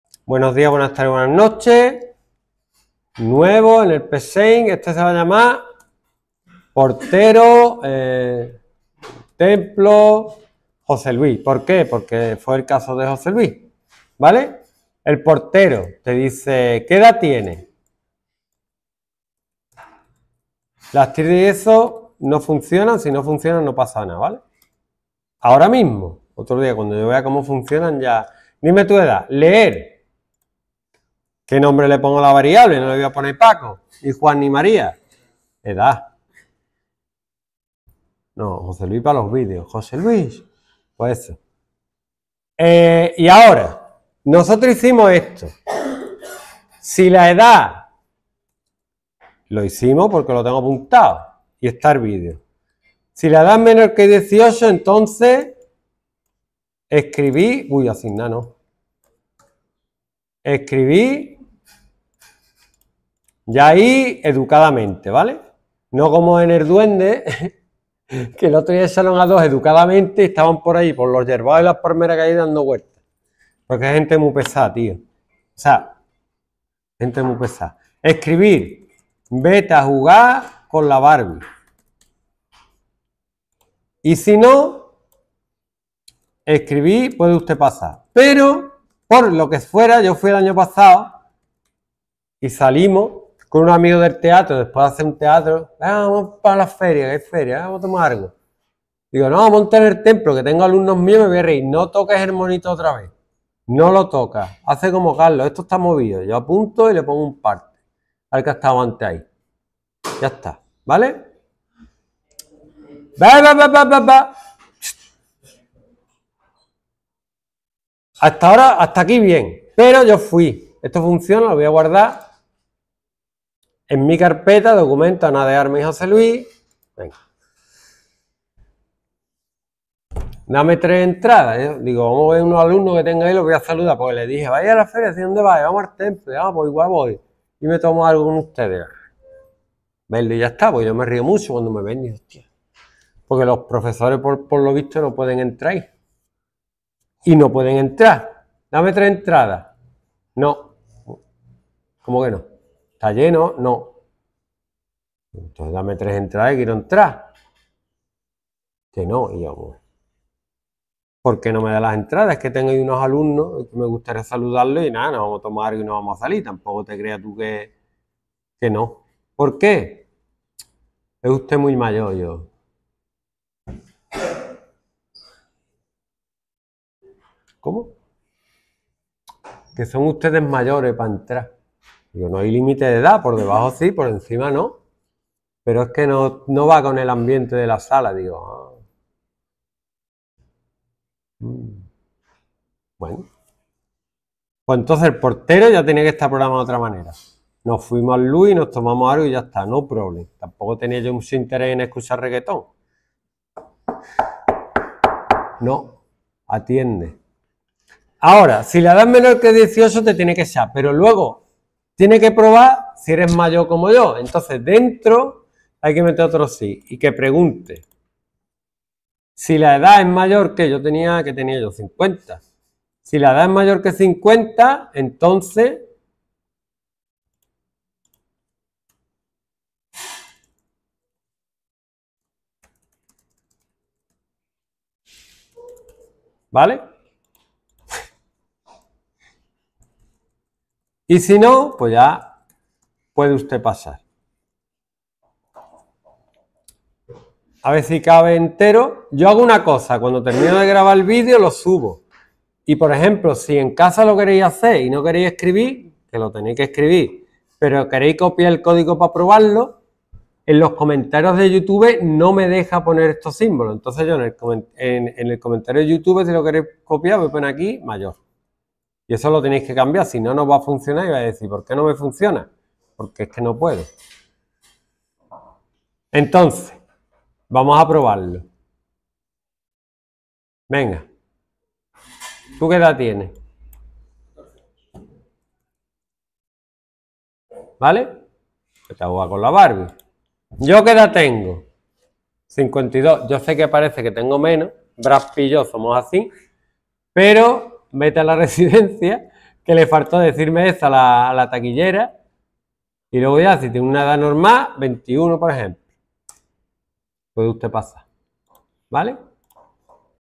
Condiciones múltiples Edad para entrar CYR3ESOAG PSEINT CYR ESO Bachillerato IES Monterroso
Algoritmo porteroTemploJL
 Escribir "Dime tu edad"
 Leer edad
 Si edadMENOR18 Entonces
  Escribir "Vete a jugar con la Barbie"
 SiNo
  Si edadMAYOR50 Entonces
   Escribir "Vete a la residencia"
  SiNo
   Escribir "Puede usted pasar"
  Fin Si
 Fin Si
FinAlgoritmo